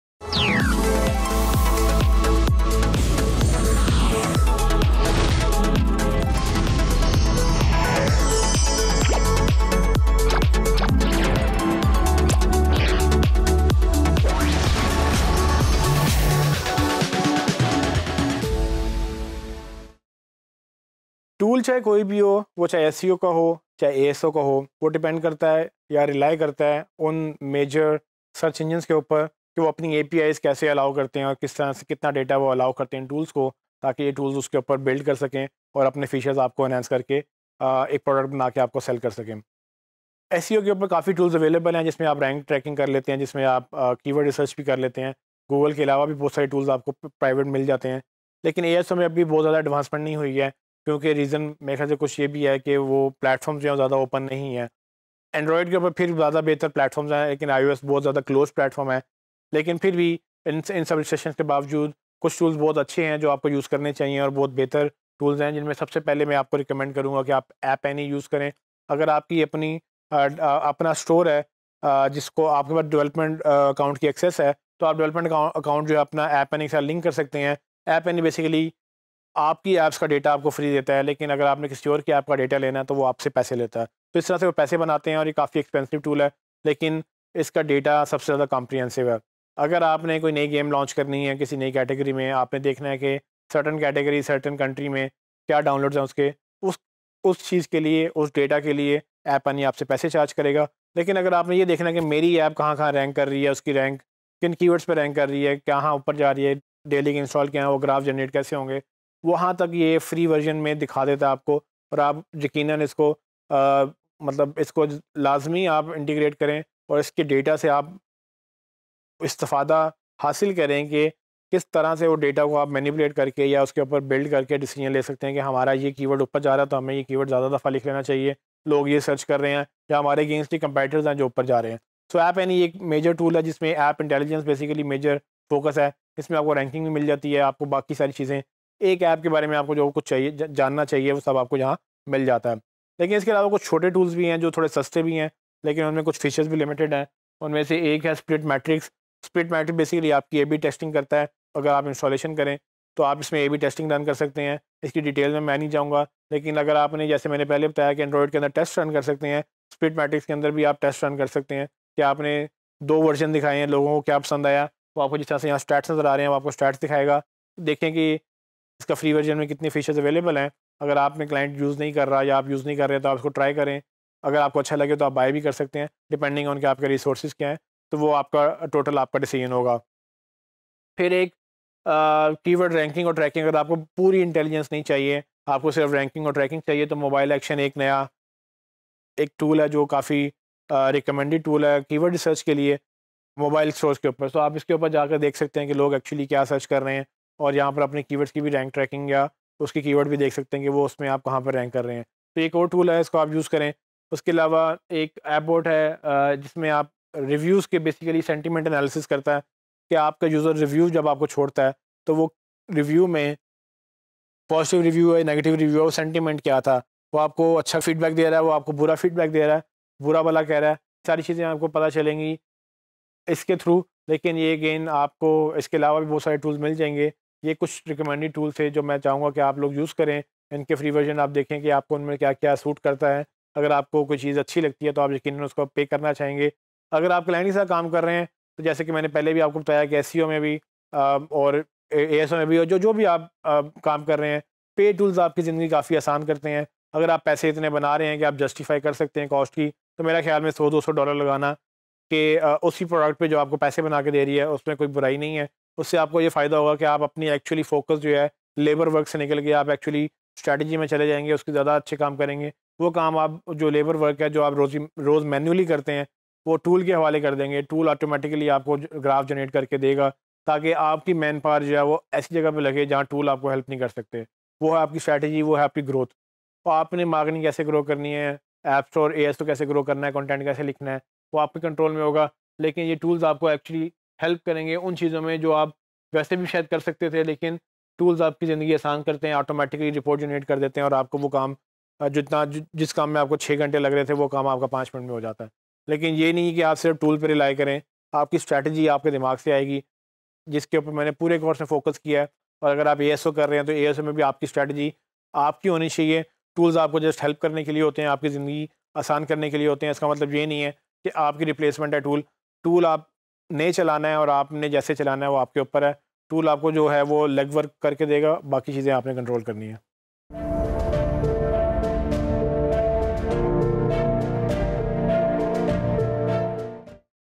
SEO#198 | Advanced SEO | Mobile SEO | Popular tools for ASO
Topic Brief

You will have an overview of top ASO tools available in the market.